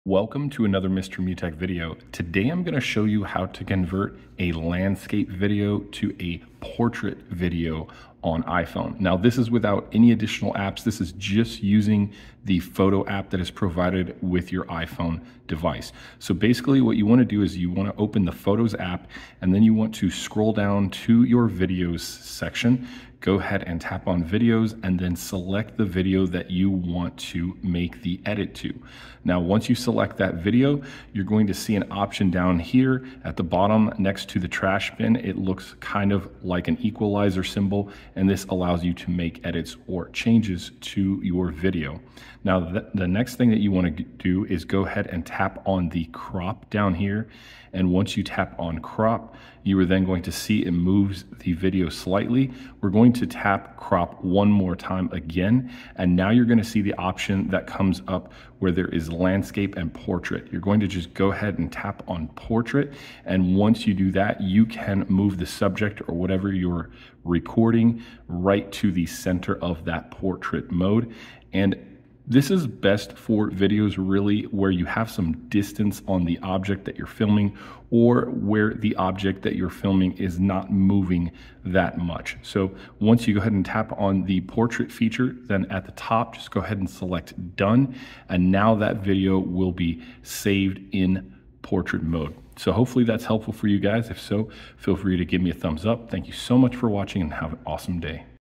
iOS 18: How To Convert Landscape Video To Portrait Video On iPhone!
Here is how to convert a landscape (horizontal) video to portrait (vertical) video on iPhone with iOS 18 in just a few steps.